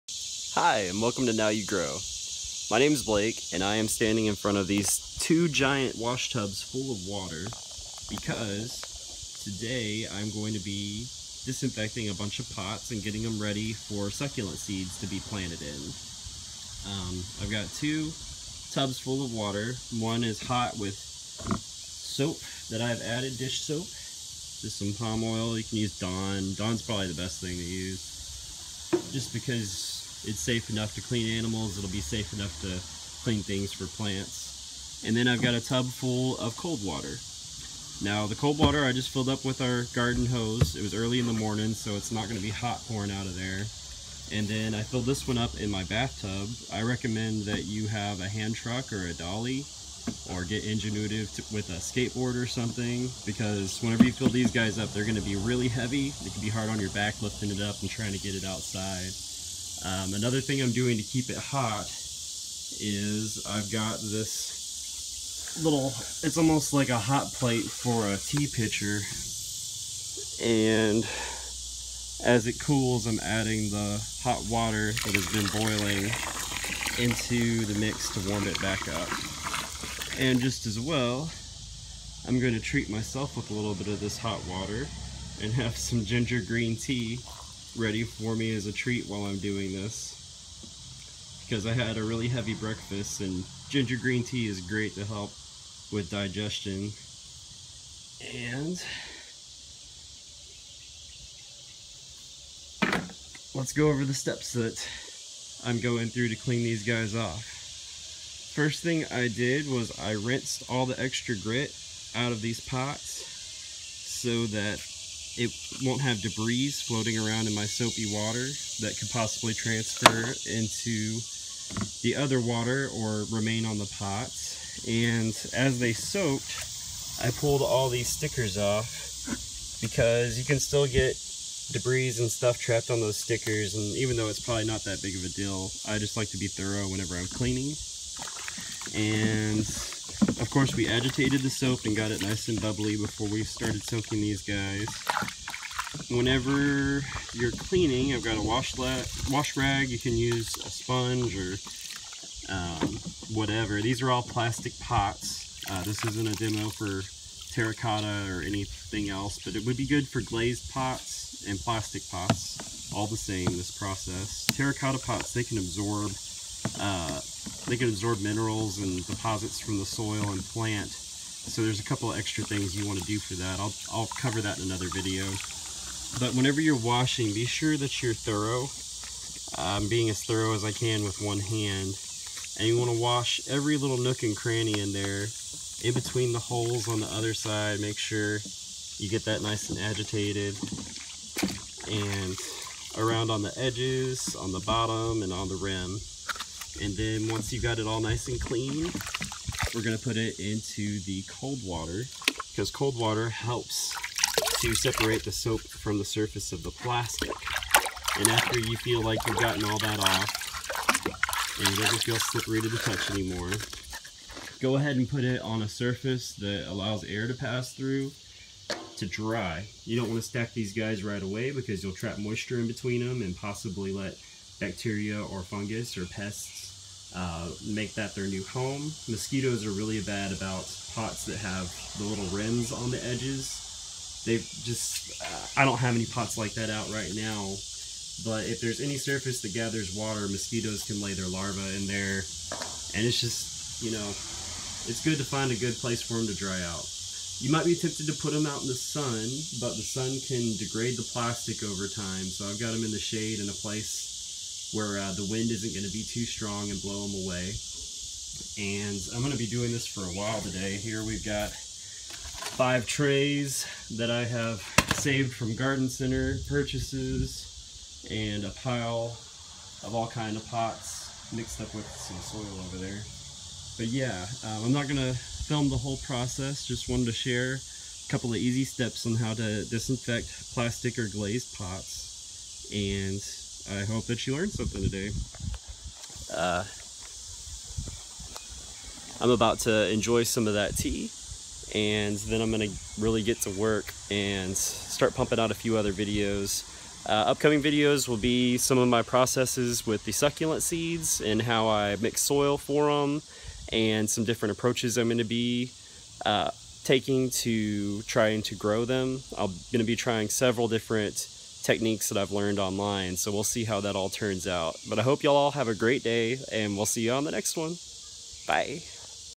How to Clean Recycled Plastic Plant Pots
Clean Recycled Plastic Plant Pots the Right Way!

Materials needed:
2 tubs that can hold water
Access to both cold and hot water
Dish soap
Wash cloth or sponge
A shady place to dry pots